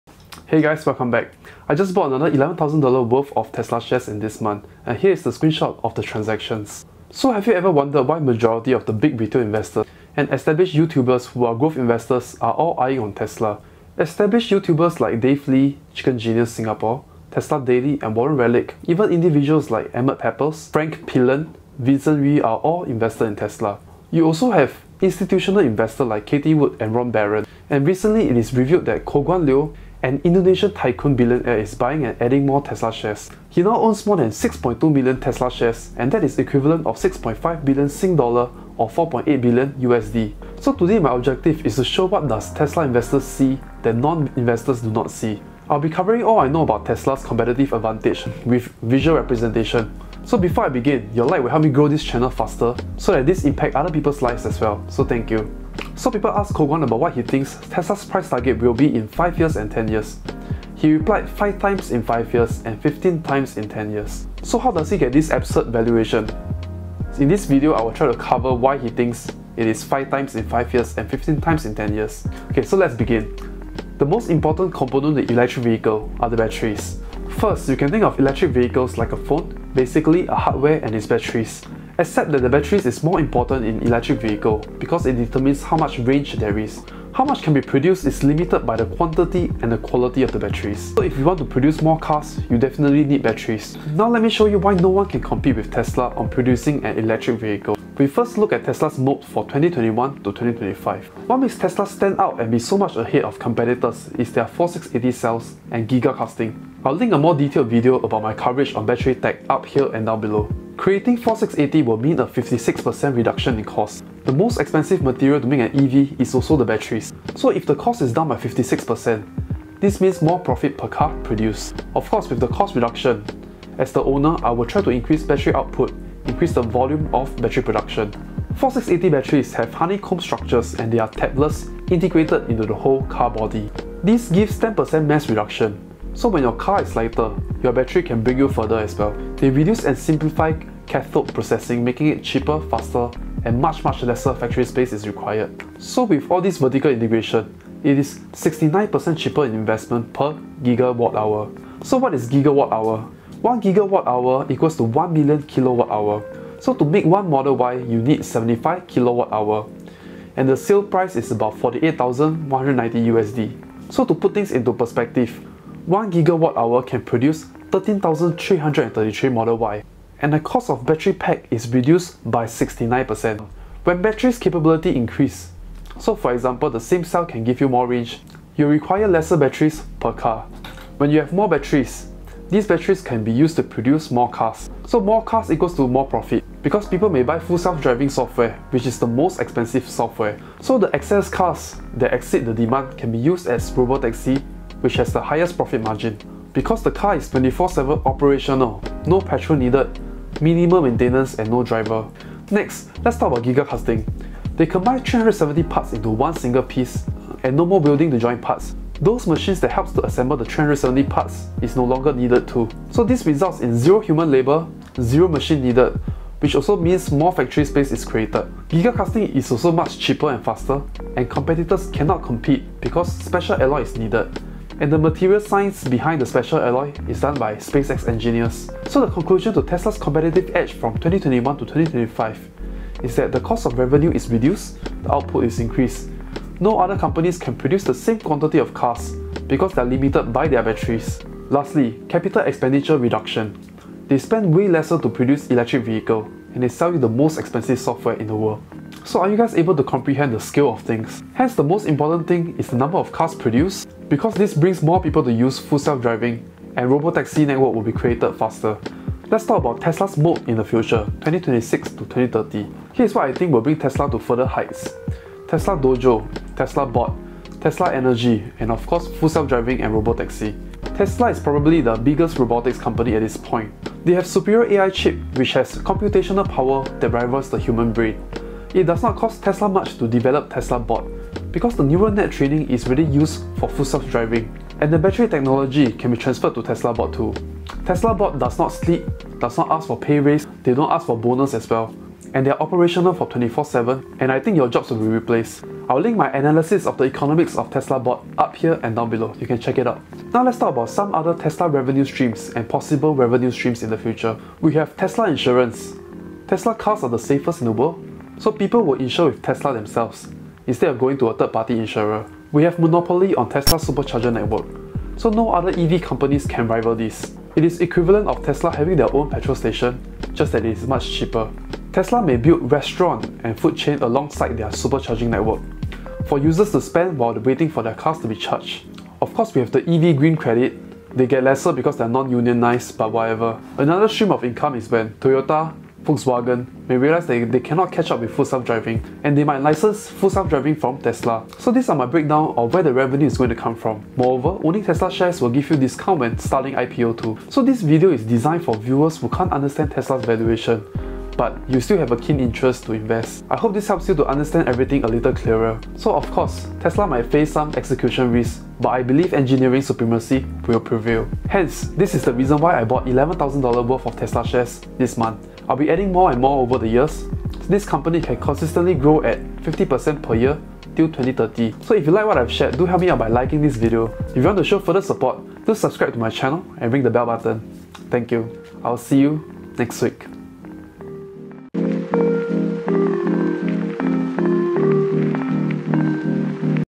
I Bought Another $11,000 TESLA Shares in 1 Month
This week I'll be sharing why I have so high conviction adding another $11,000 worth of Tesla shares in this month alone.

I will be sharing what is Tesla's competitive advantage for (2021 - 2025)
I will be covering Tesla's competitive advantage for (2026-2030) as well.

This video is design for viewers who can't wrap their head around Tesla's valuation.

I will be explaining with visual representation